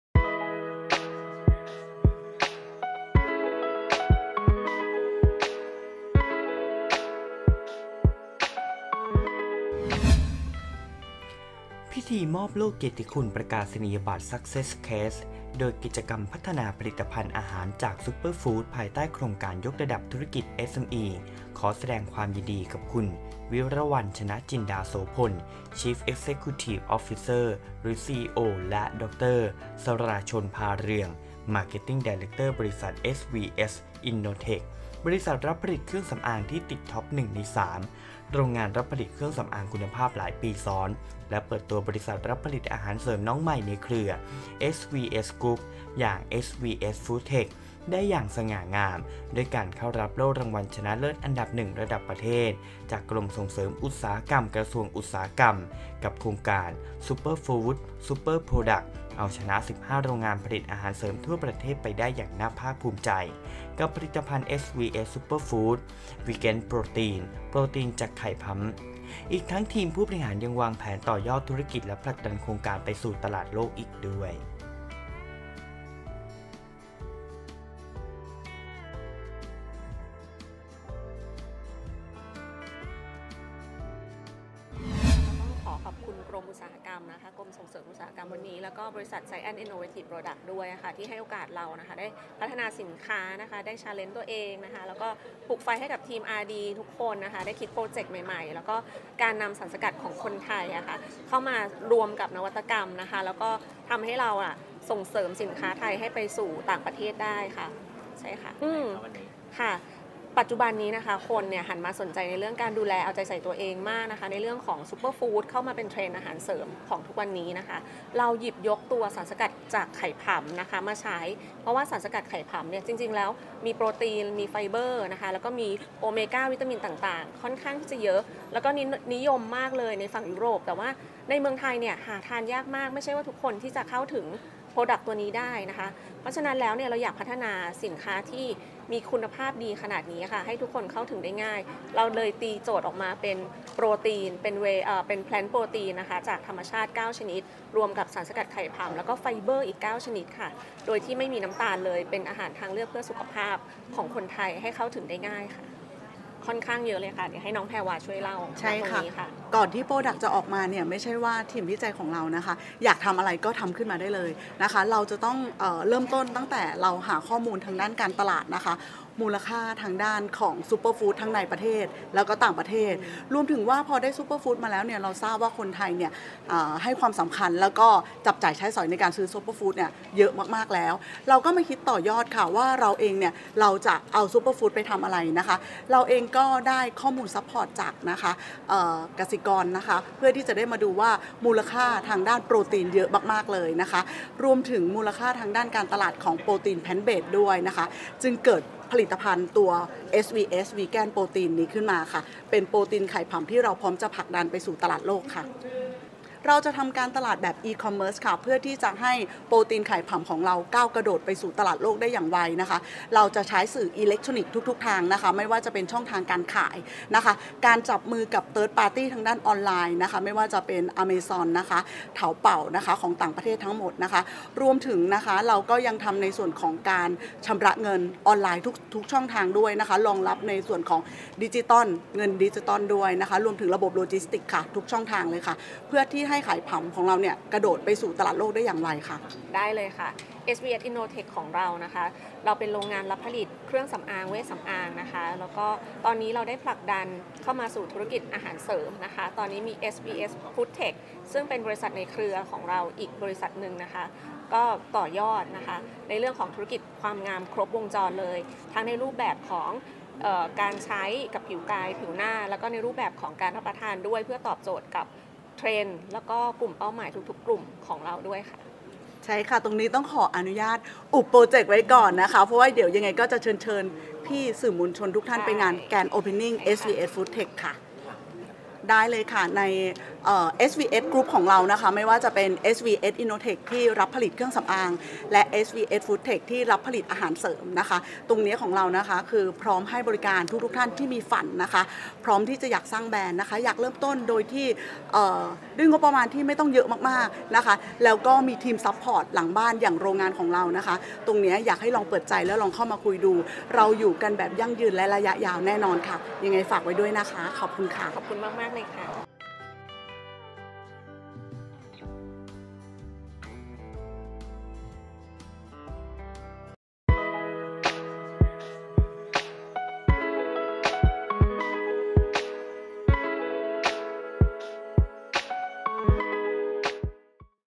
สุดยอดนวัตกรรม บริษัท SVS INNOTECH คว้าโล่รางวัลชนะเลิศอันดับ 1 ระดับประเทศ
สุดยอดนวัตกรรม บริษัท SVS INNOTECH คว้าโล่รางวัลชนะเลิศอันดับ 1 ระดับประเทศ จากกรมส่งเสริมอุตสาหกรรม กระทรวงอุตสาหกรรมกับโครงการ SUPERFOOD  SUPERPRODUCT

พิธีมอบโล่เกียรติคุณ ประกาศนียบัตร Success Case
โดยกิจกรรมพัฒนาผลิตภัณฑ์อาหารจาก ซุปเปอร์ฟู้ด ภายใต้โครงการยกระดับธุรกิจ SMEs
ขอแสดงความยินดีกับ คุณวิรวรรณ ชนะจินดาโสภณ Chief Executive Officer (CEO) และดร.สราชล พาเรือง Marketing Director บริษัท SVS INNOTECH บริษัทรับผลิตเครื่องสำอางที่ติดTop 1 ใน 3 โรงงานรับผลิตเครื่องสำอางคุณภาพหลายปีซ้อน และเปิดตัวบริษัทรับผลิตอาหารเสริมน้องใหม่ในเครือ SVS GROUP อย่าง SVS FOODTECH ได้อย่างสง่างามด้วยการเข้ารับโล่รางวัลชนะเลิศอันดับ 1 ระดับประเทศจากกรมส่งเสริมอุตสาหกรรม  กระทรวงอุตสาหกรรม กับโครงการ SUPERFOOD  SUPERPRODUCT เอาชนะ 15 โรงงานผลิตอาหารเสริมทั่วประเทศไปได้อย่างน่าภาคภูมิใจกับผลิตภัณฑ์ SVS SUPERFOOD VEGAN PROTEIN (โปรตีนจากไข่ผำ) อีกทั้งทีมผู้บริหารยังวางแผนต่อยอดธุรกิจและผลักดันโครงการไปสู่ตลาดโลกอีกด้วย